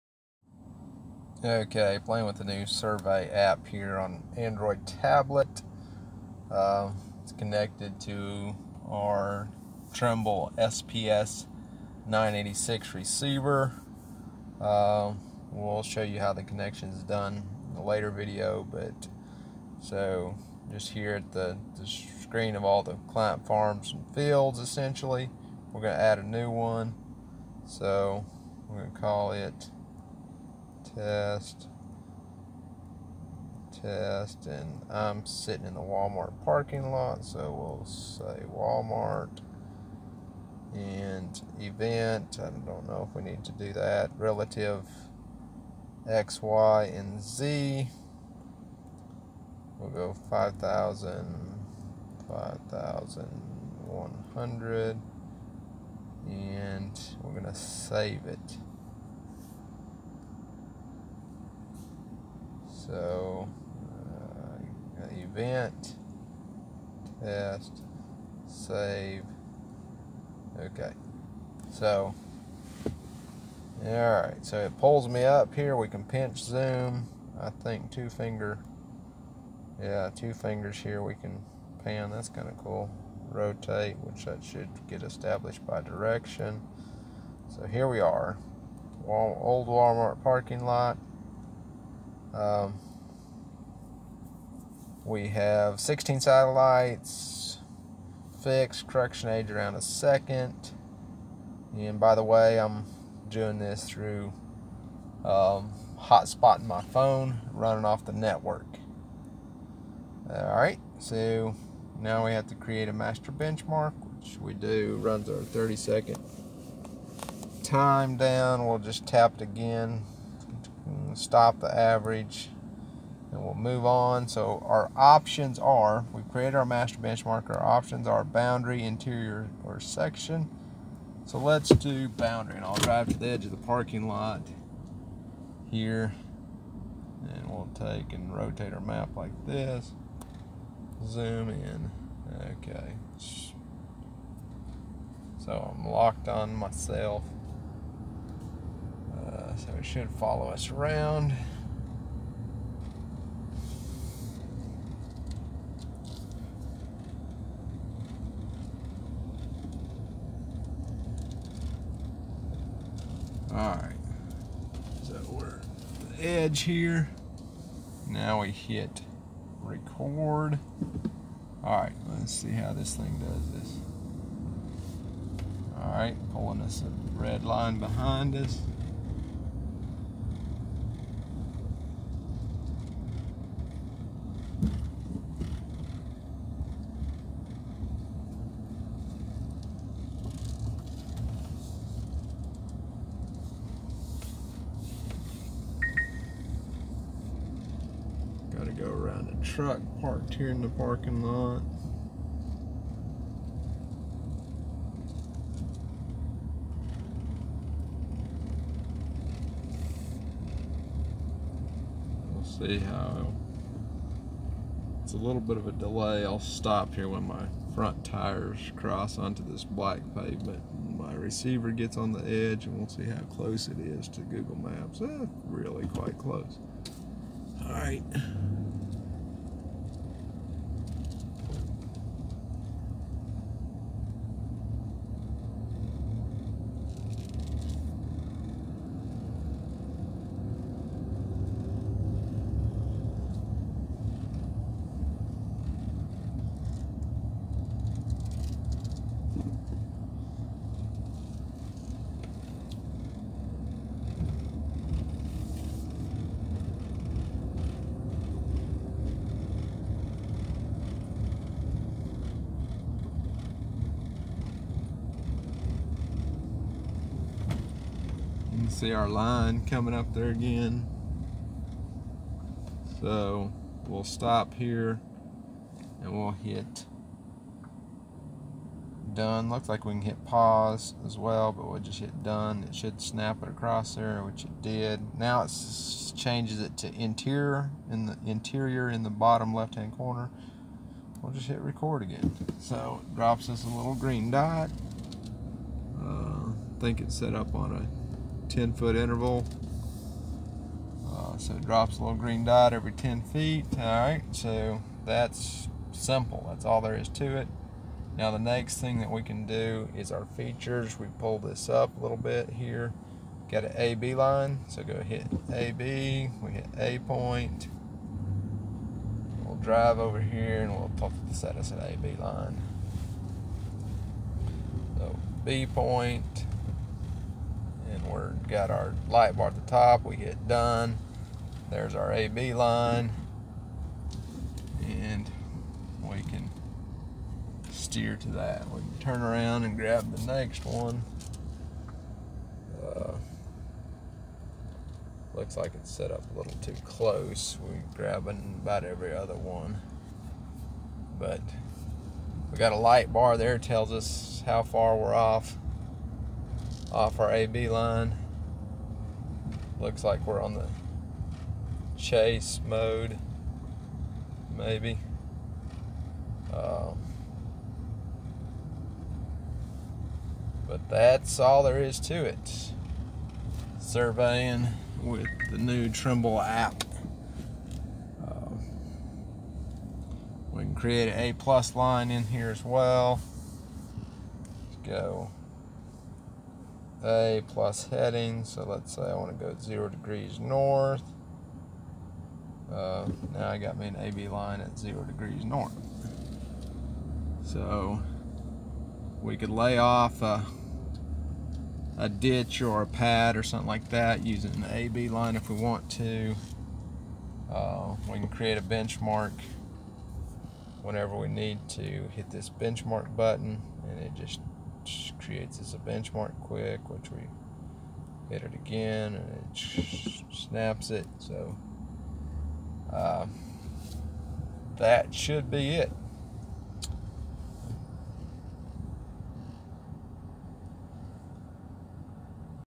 Trimble WM Survey App
Trimble WM Survey App walk through using Trimble SPS 986 receiver.